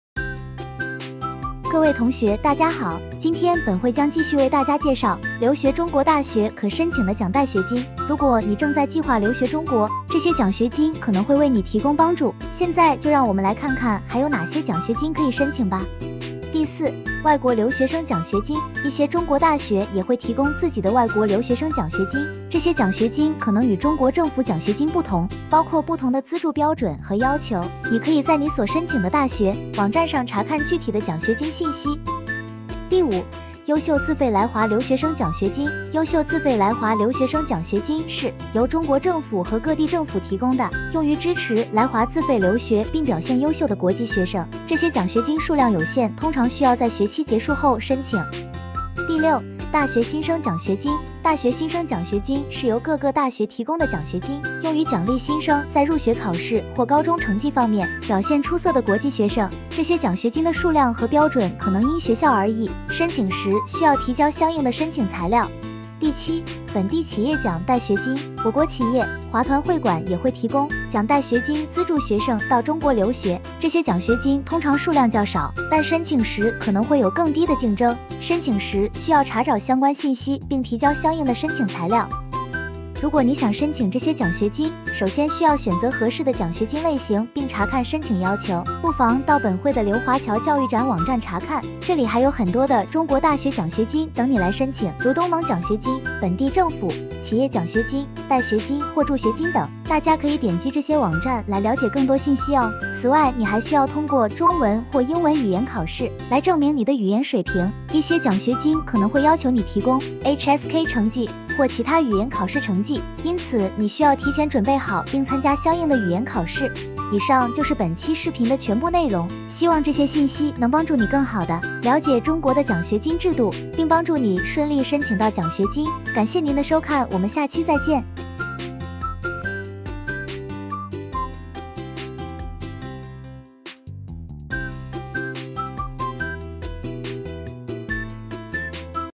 留学中国奖贷学金资讯 part 2【留学中国小贴士】- by 马来西亚留华同学会
申请中国大学可申请什么类型的奖贷学金呢？ 本期视频就来告诉你！
想知道更多资讯记得留守本会的【留学中国小贴士】系列！